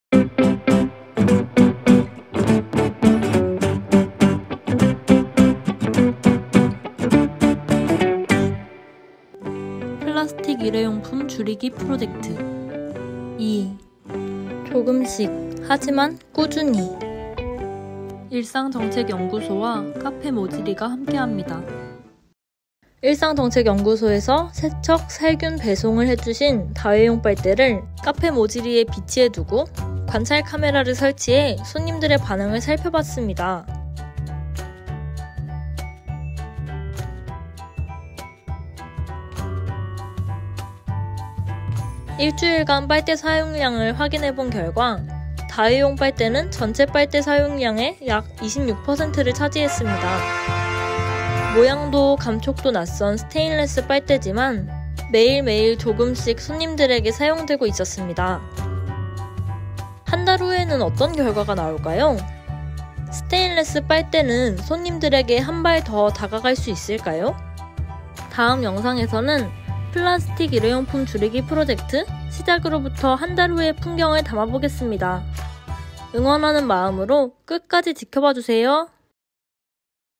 친환경 빨대 실험! 두번째 이야기 : 조금씩, 하지만 꾸준히
일상정책연구소는 2030 청년들이 모여 부천지역의 사회문제에 대한 정책 제안 및 사회적 운동을 기획하는 단체인데요! 이번에 카페 모지리와 함께 플라스틱 일회용품 줄이기 프로젝트를 하게 되었어요.
   카페 모지리는 8월 말부터 스테인레스로 다회용 빨대와 손수건을 늘 비치하고 있습니다. 프로젝트에 사용되는 다회용 제품들은 모두 일상정책연구소에서 세척, 살균, 배송을 해주신답니다. 많은 분들이 다회용 제품의 청결에 대해 우려하시지만 걱정은 no no! 스테인레스 빨대와 손수건은 끓는 물로 살균이 가능하기 때문에 청결을 믿고 사용하셔도 된답니다~
   두번째 이야기 : 조금씩, 하지만 꾸준히 에서는 모지리에서 일어나고 있는 작은 변화의 모습을 담았습니다. 관찰카메라를 설치하여 일주일간 관찰한 결과, 다회용 빨대 사용이 총 빨대 사용의 26%를 차지하였습니다. 한달이 지난 후에는 어떤 변화가 우리를 기다리고 있을까요? 다음 영상도 지켜봐주세요!